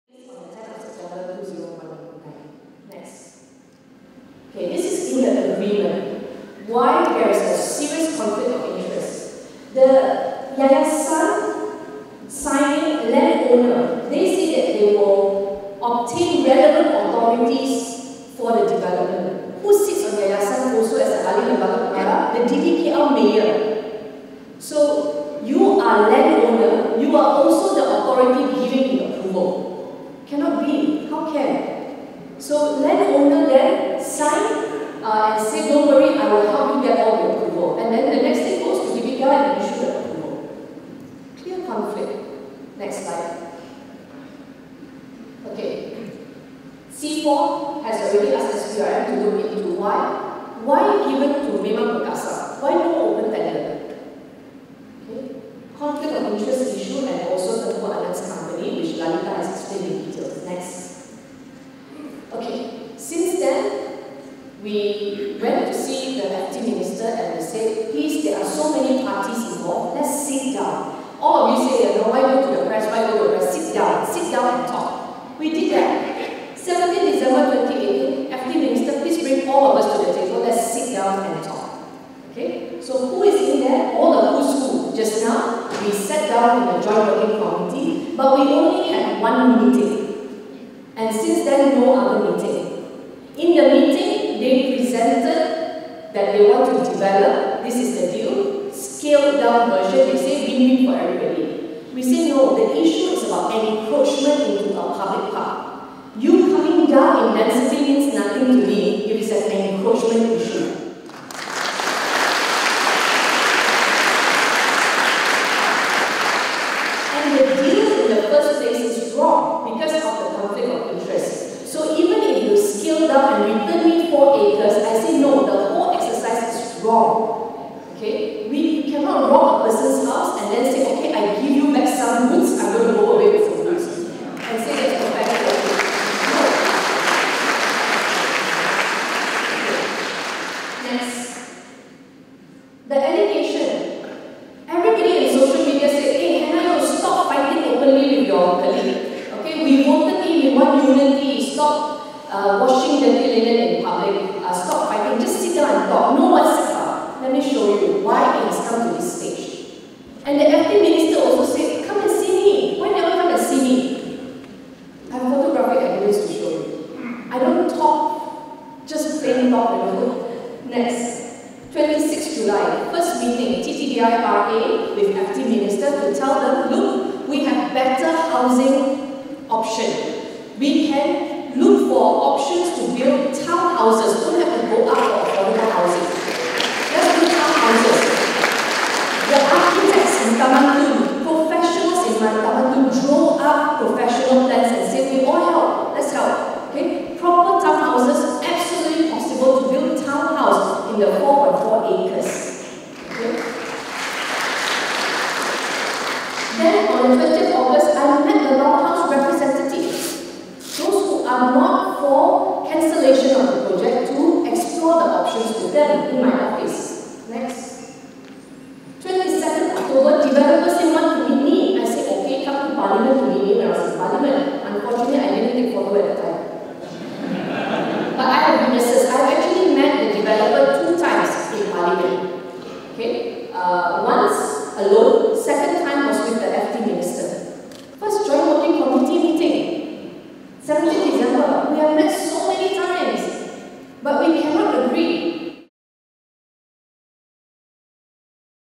Yeoh shows proof of meetings with Khalid over Taman Rimba Kiara project issue
Segambut MP Hannah Yeoh said she had met Federal Territories Minister Khalid Samad several times over a housing project issue in Taman Rimba Kiara but failed to come to any agreement.

At a briefing with residents on Saturday, Yeoh said this in response to Khalid's comment on May 16 about her going through the media instead of discussing the matter with the ministry.